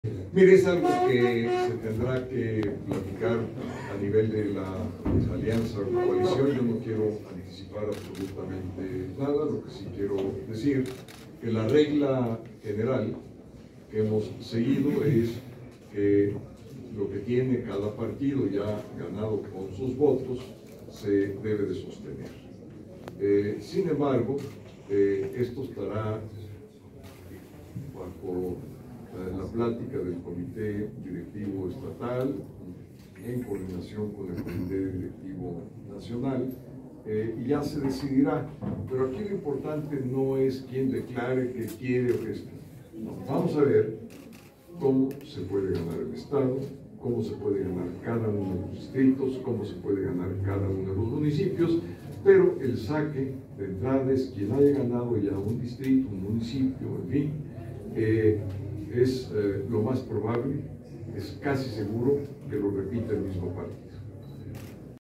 Los resultados del PRI, PAN y el PRD definirán las candidaturas de Va por México en Puebla: Creel
Los resultados electorales del PRI, PAN y el PRD definirán la distribución de candidaturas de la alianza Va por México en Puebla: Santiago Creel